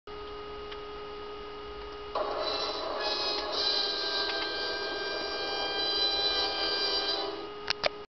Dramatic Headless Snowman
This is my first dramatic chipmunk parody video.